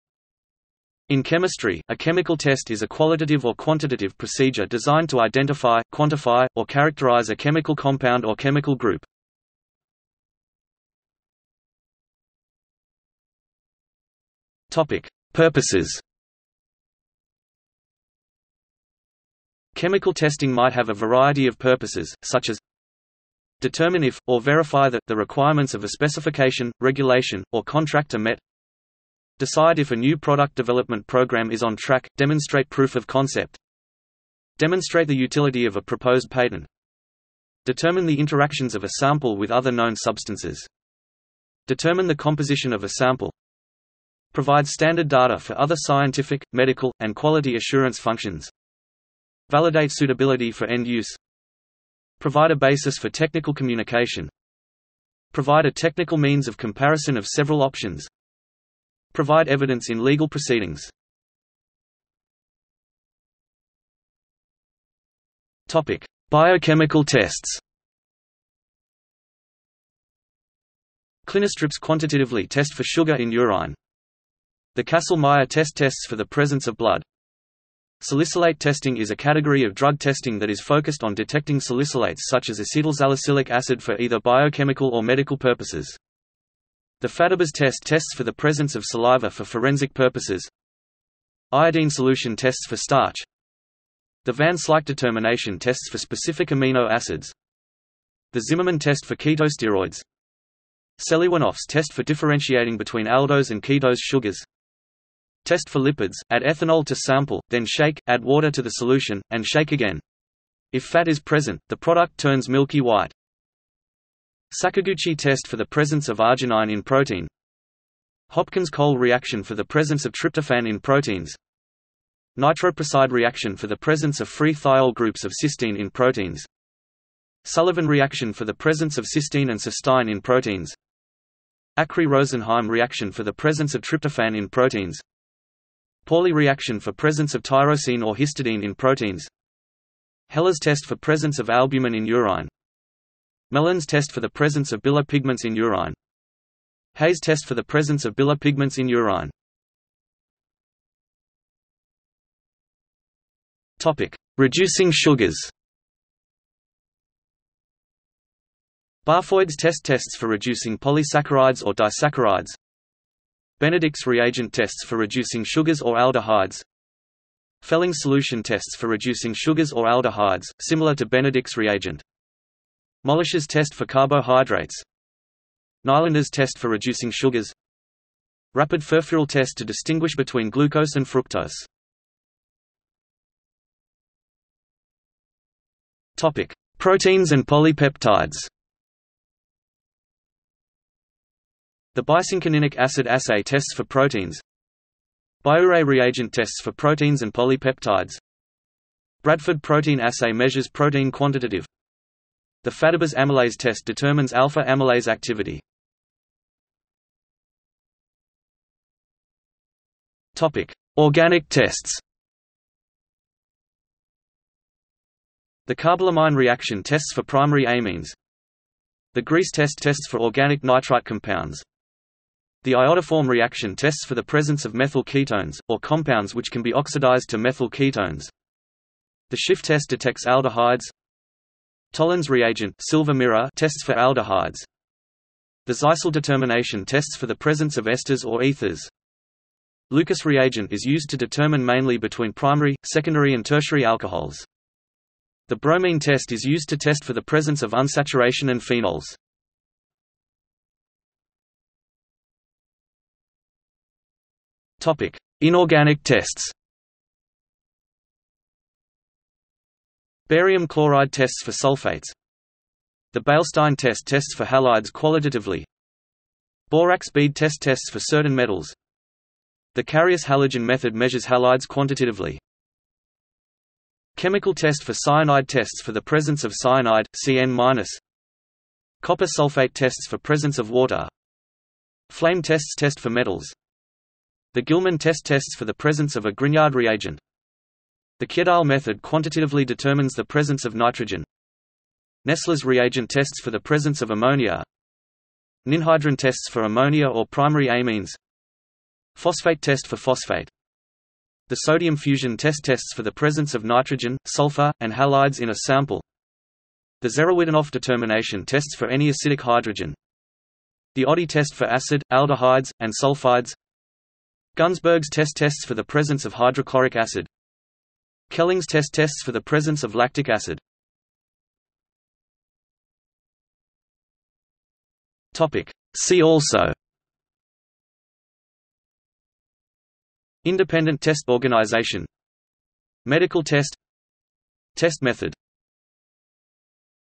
Chemical test | Wikipedia audio article
00:00:10 1 Purposes
00:01:01 2 Biochemical tests
00:02:30 2.1 Reducing sugars
00:03:02 2.2 Proteins and polypeptides
00:03:26 3 Organic tests
00:04:11 4 Inorganic tests
00:05:21 5 See also

- Socrates

SUMMARY
In chemistry, a chemical test is a qualitative or quantitative procedure designed to identify, quantify, or characterise a chemical compound or chemical group.